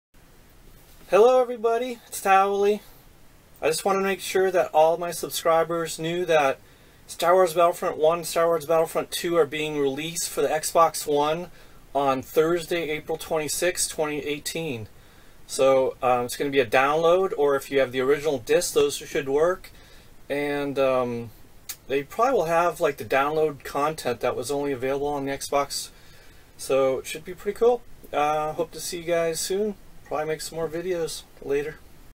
Star Wars Battlefront 1 & 2 for Xbox One on April 26th, 2018 download backwards compatible
The original Star Wars Battlefront (2004) and SWBF 2 (2005) are coming to Xbox One as downloads.  If you have the original disks, they should work.  Thank you to Andy1341000 for letting me know about this.  MULTIPLAYER AND THE ADDITIONAL MAPS ARE NOT AVAILABLE FOR THESE TWO GAMES.